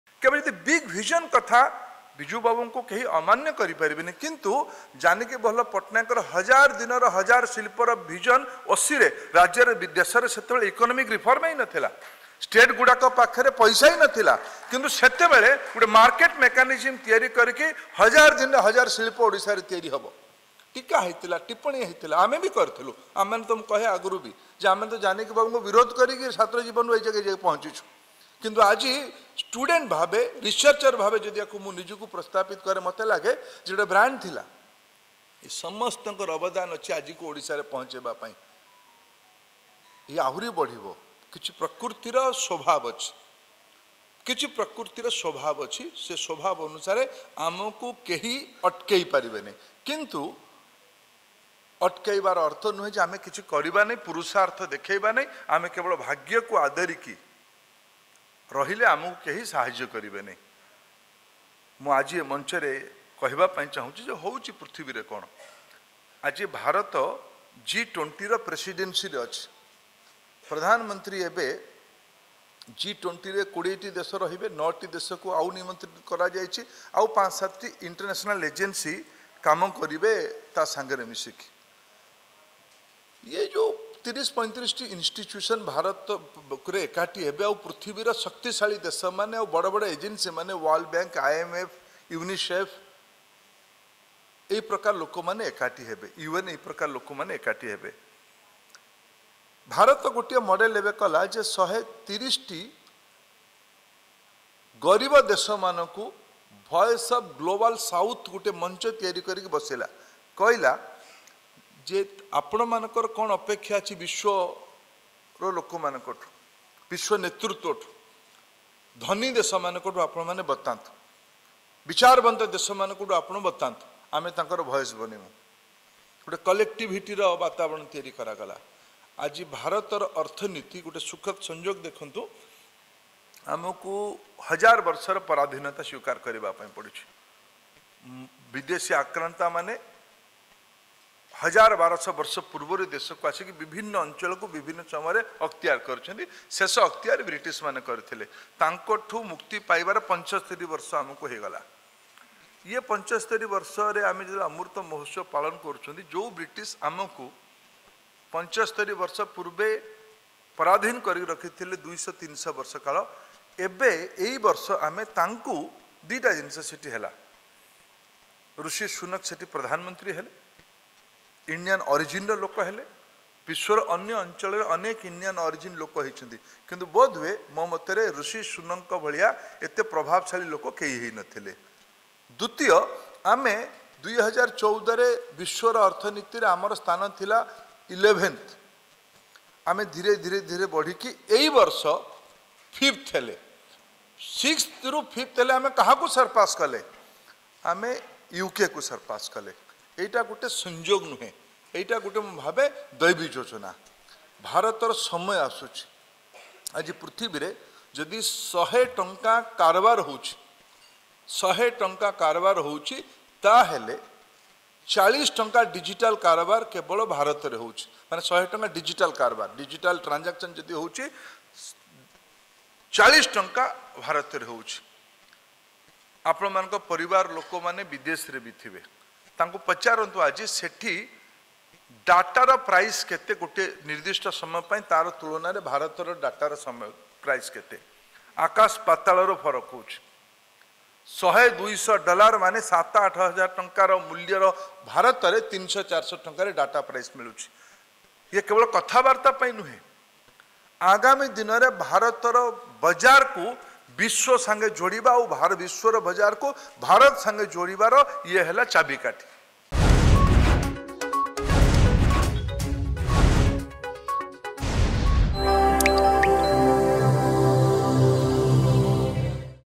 India's Economy Has Overtaken UK's Economy To Become 5th In The World : Union Minister Pradhan
India's Economy Has Overtaken UK's Economy To Become 5th In The World : Union Minister Dharmendra Pradhan